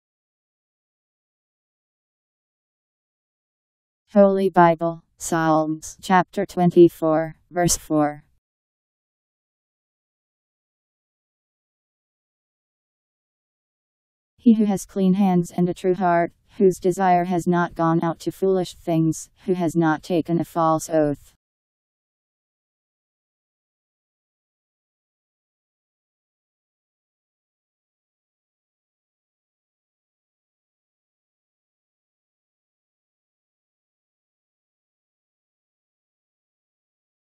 Holy Bible Psalms 24:4
Video shows Holy Bible Psalms Chapter 24 Verse 4: He who has clean hands and a true heart; whose desire has not gone out to foolish things, who has not taken a false oath. Powered by MaryTTS. Text taken from Bible in Basic Edition. Bible Psalms 24:4